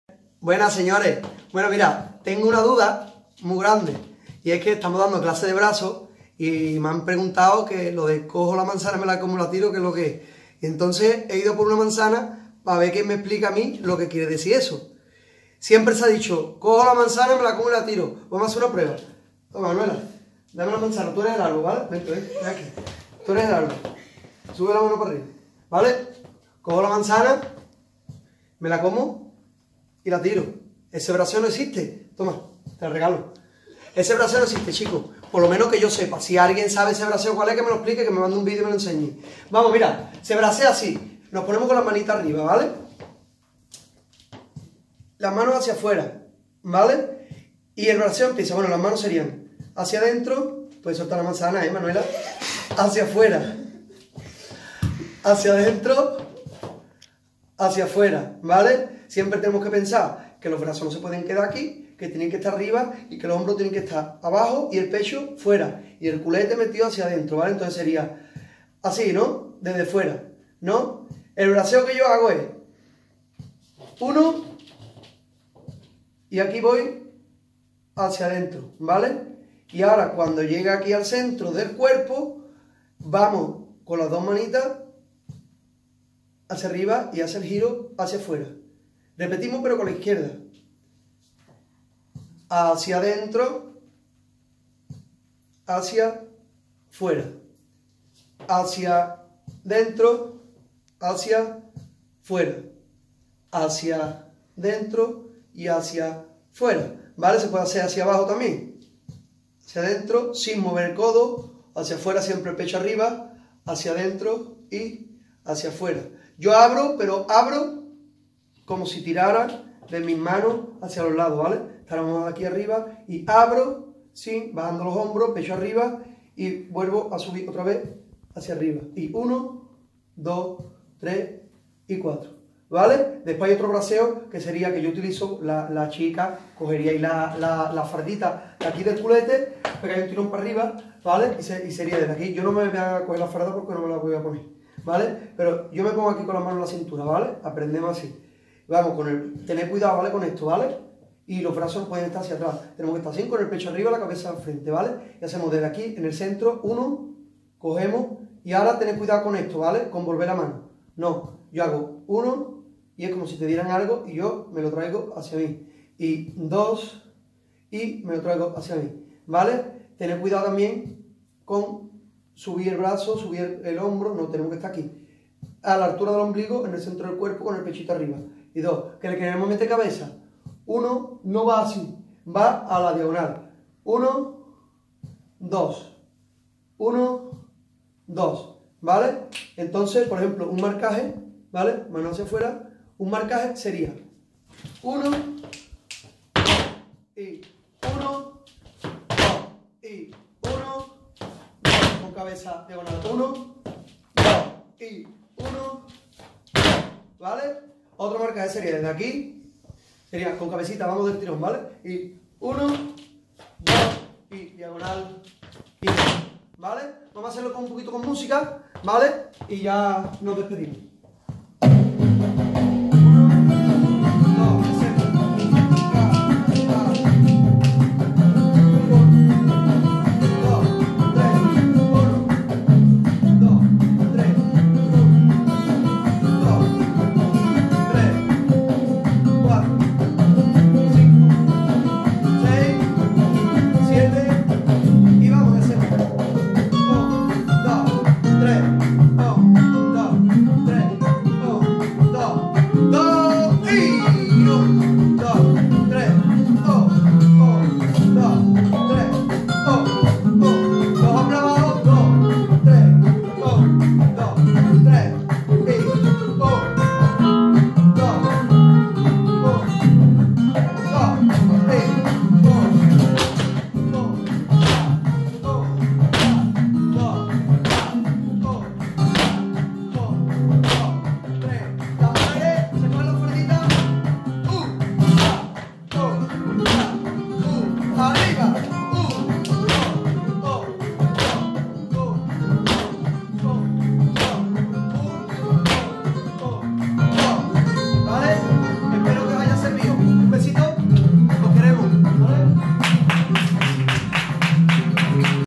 ¿Cojo la manzana, me la como y la tiro...? Clase de técnica de brazos
Clase de técnica de brazos flamenco
Página oficial Instagram @duartedanza
Página oficial Instagram Sergio González @sergiobailaor